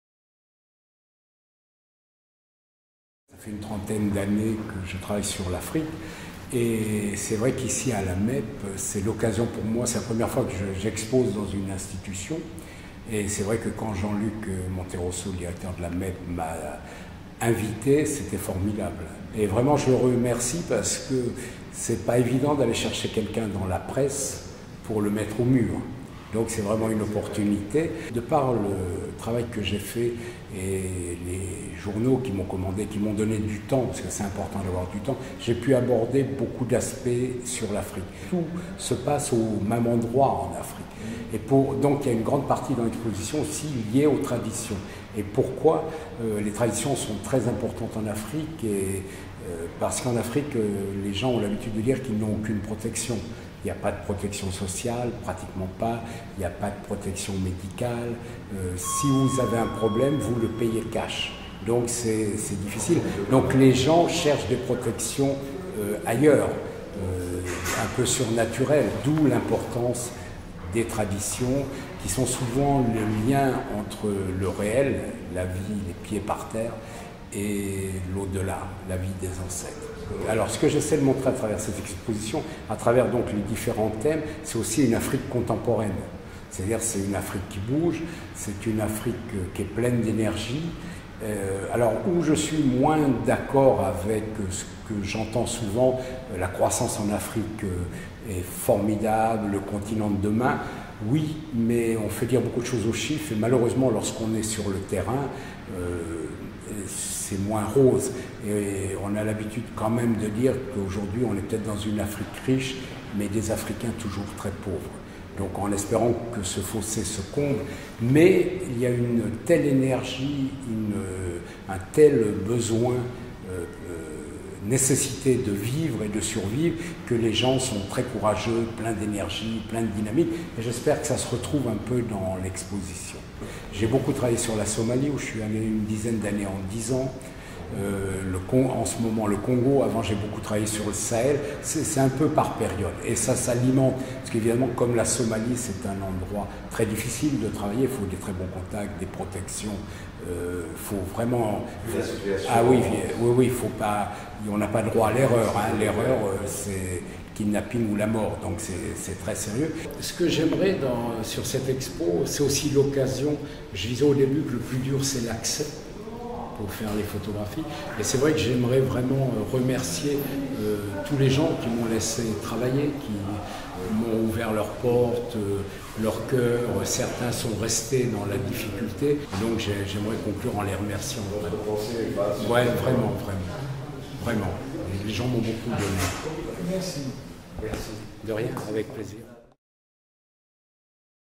Interview Pascal Maitre "Afrique(s)" à la Maison Européenne de la Photographie 2014
"Afrique(s)" vu de l'interieur par Pascal Maitre, grand reporter des pays en crise de l'Afrique au proche orient.
L'Afrique vu sous tous ces aspects. Quand la tradition se conjugue avec la création, des questions tabous sur l'intégrisme, les pirates de Somalie, etc...
Le film n'est qu'un aperçu de son travail qu'il faut absolument voir à la maison européenne de la photographie jusqu'au 2 Novembre 2014.
Film réalisé par la Maison Européenne de la Photographie.

Montage :  Anne-Sophie Jessel